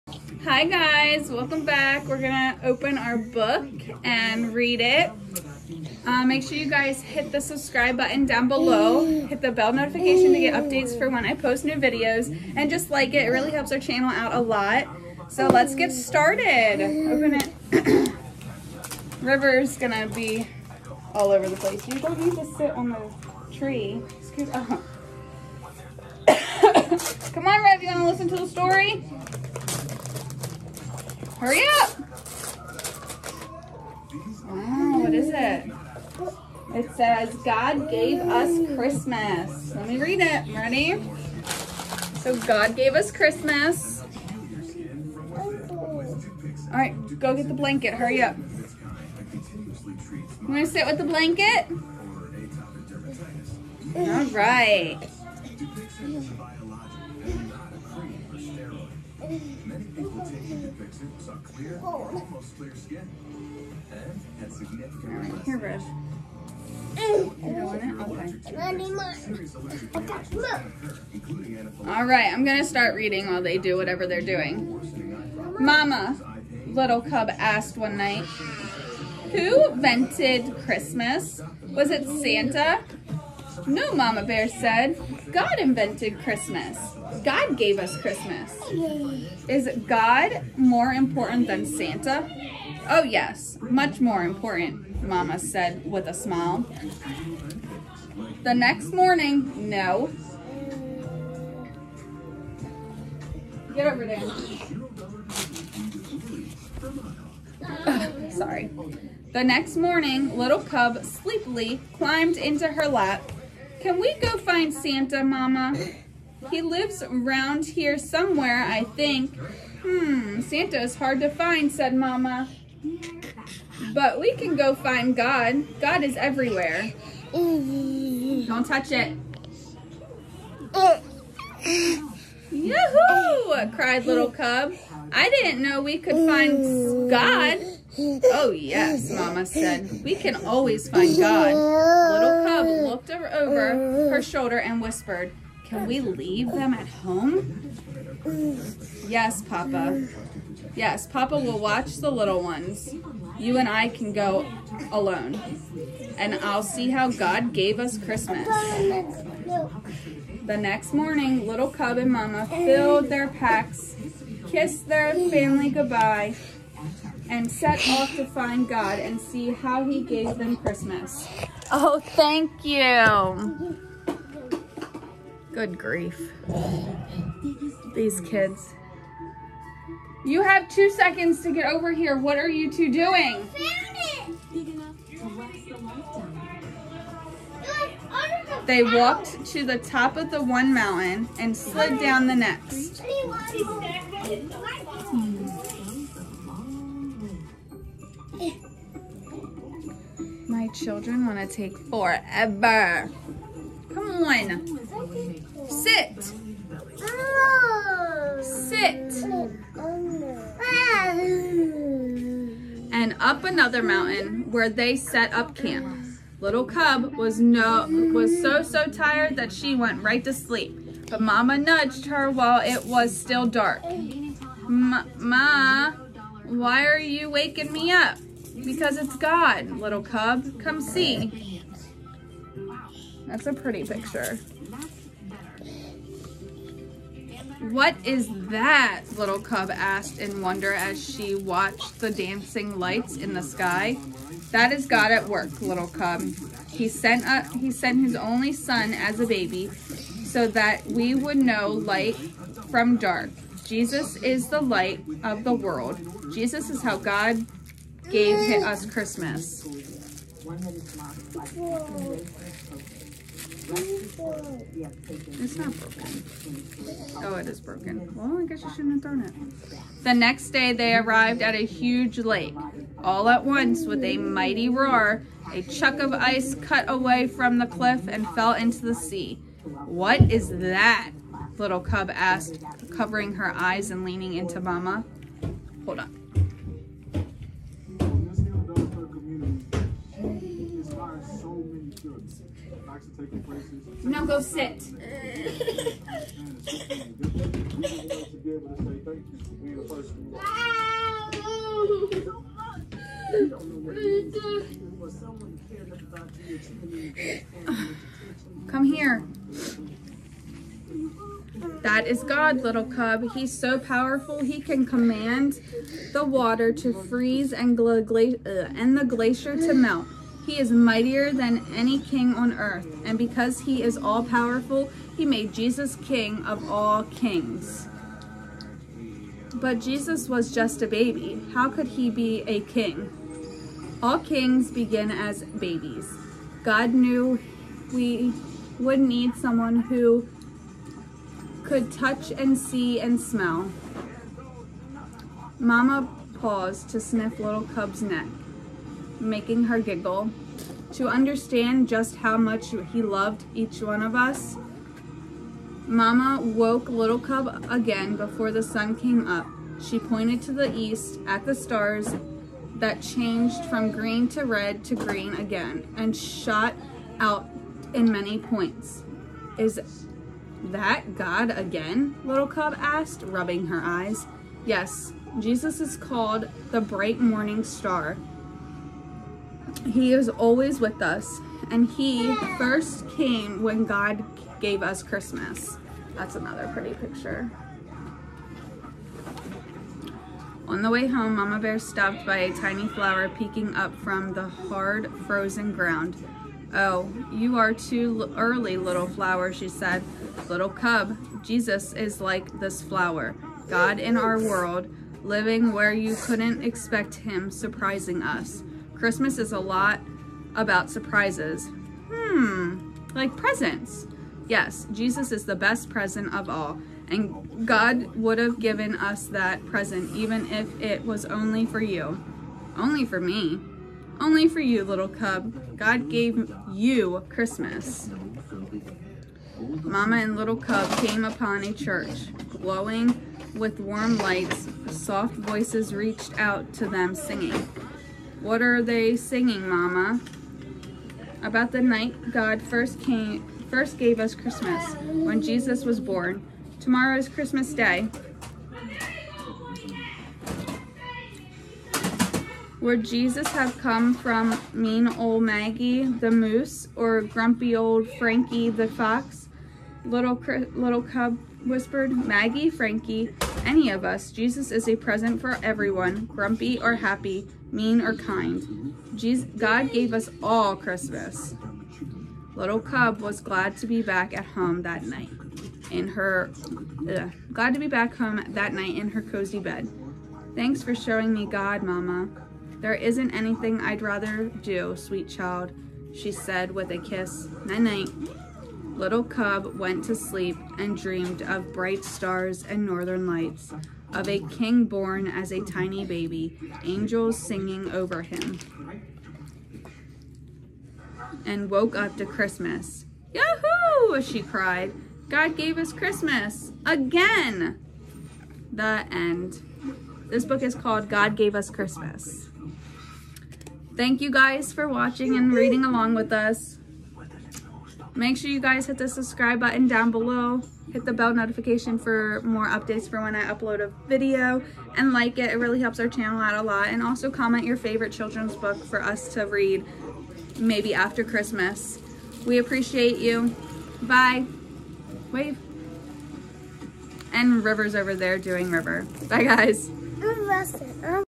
11 days til Christmas | Come read with us | God gave us Christmas
We have started a new tradition of opening a wrapped book and reading it before bed the last 12 days til Christmas. This will end on Christmas! Come read with us every night! Tonight’s book is God gave us Christmas!